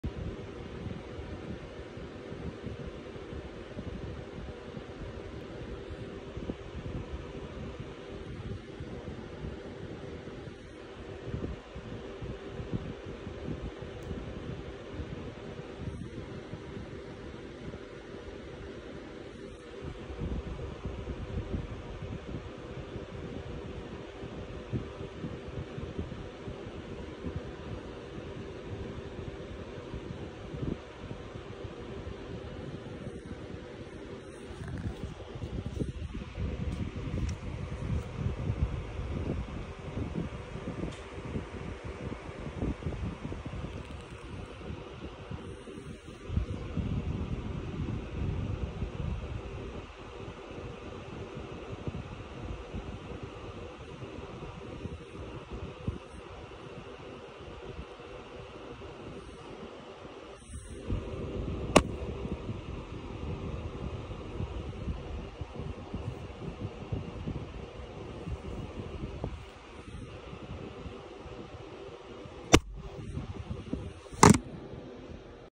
A tv remote knocks me out.
I get knocked out by a TV remote. How NORMAL?!